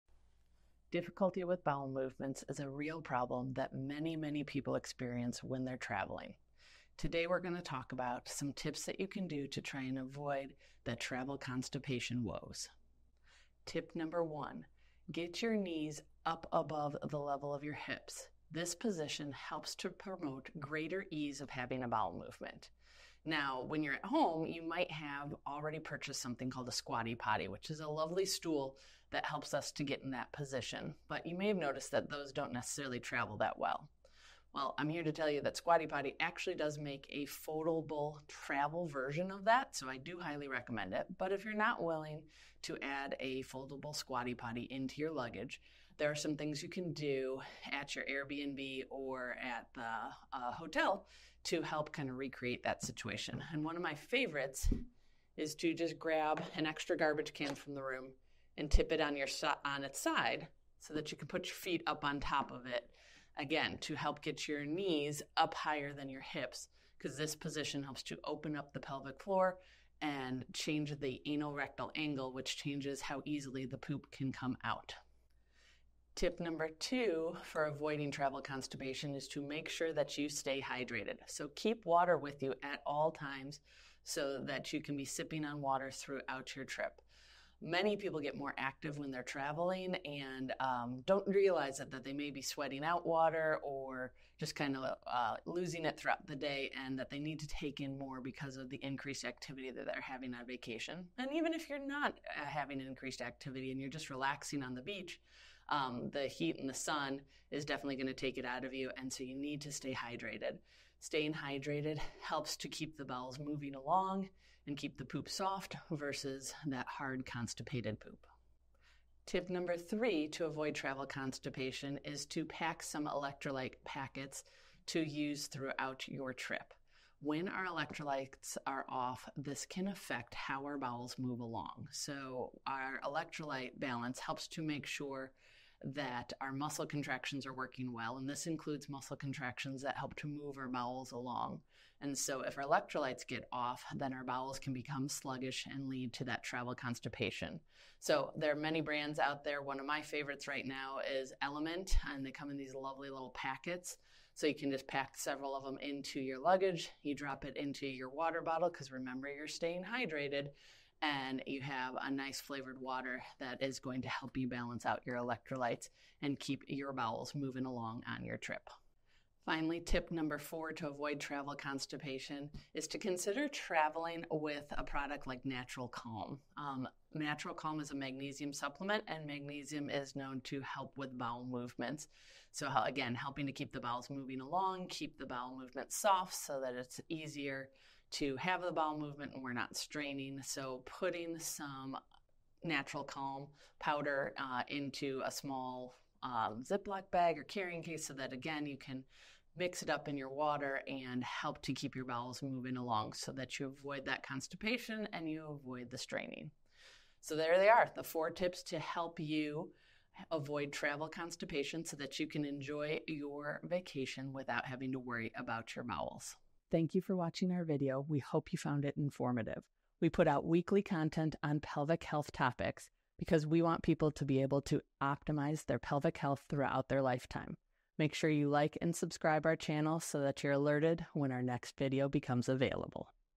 Travel Constipation Hacks
Travel constipation happens to many individuals when they are away from home. In this video we go over simple tips you can use to avoid getting constipated on your next trip.

🕒 Timestamps
00:00 - Introduction: Avoiding Travel Constipation
00:30 - Tip #1: Get Your Knees Above Your Hips (Squatty Potty & Travel Hacks)
01:16 - Tip #2: Stay Hydrated (Importance of Water While Traveling)
02:06 - Tip #3: Pack Electrolyte Packets for Better Bowel Function
02:49 - Tip #4: Use Magnesium Supplements like Natural Calm
03:27 - Recap: Four Tips to Avoid Travel Constipation